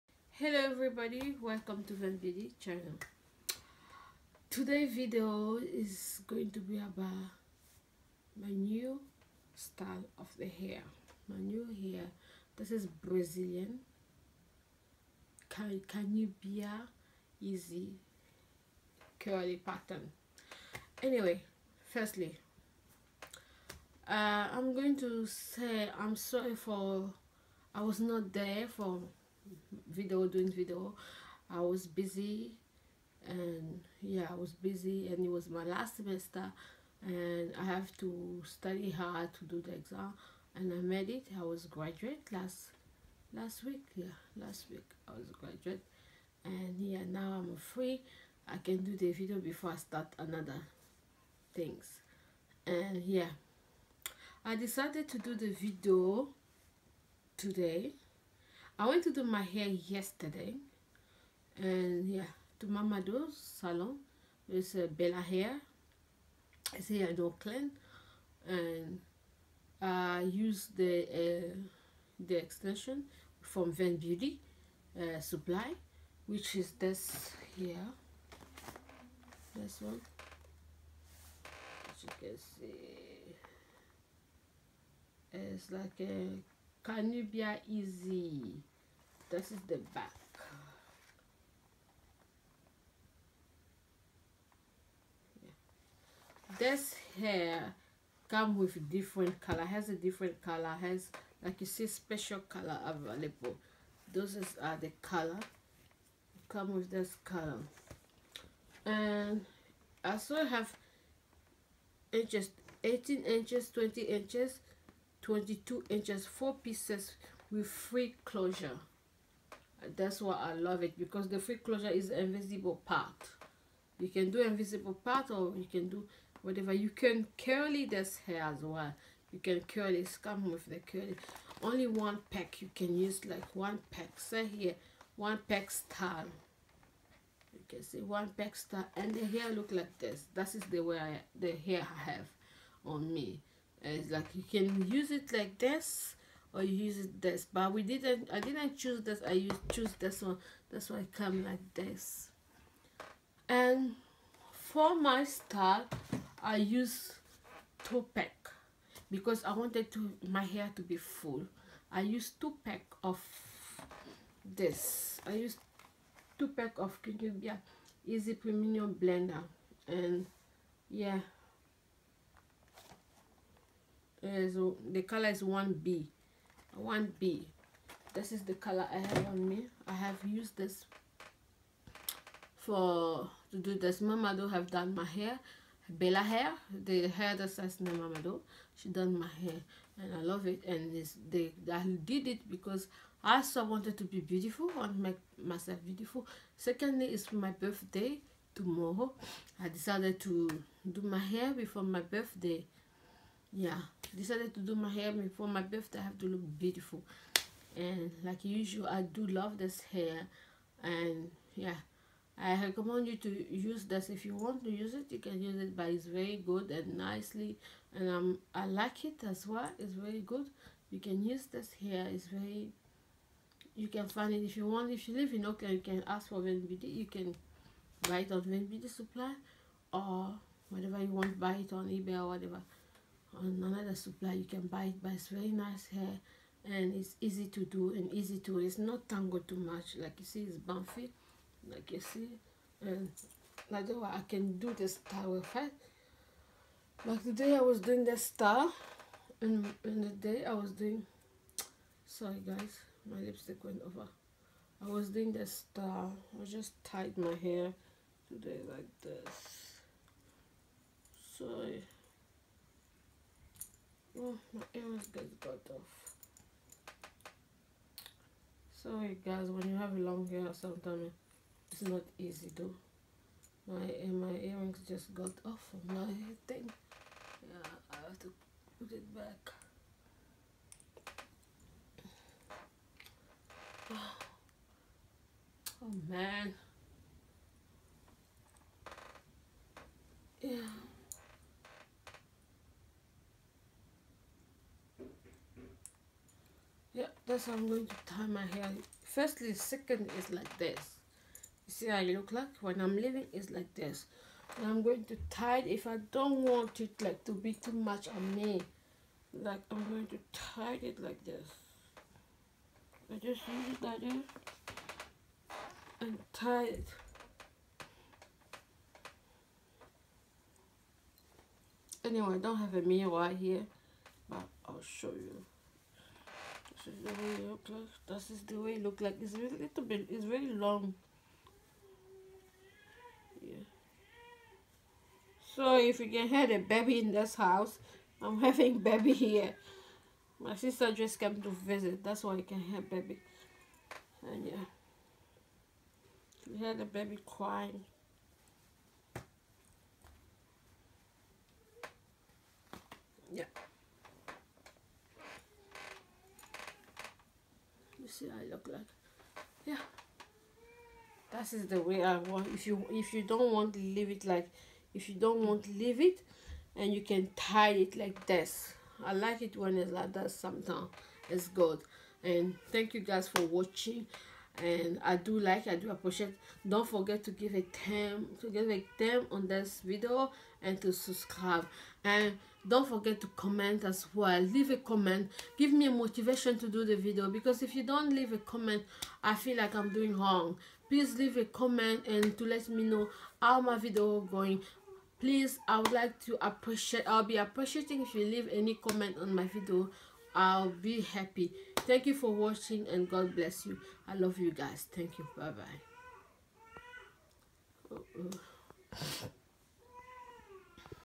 Brazilian hair curl patterns(Kanubia Easy5)
Also I wanted  to look beautiful for my birthday , anyway This hair is good  quality  also it's come with is closure,  but if you need this you buy at Vhen beauty supply jus email us.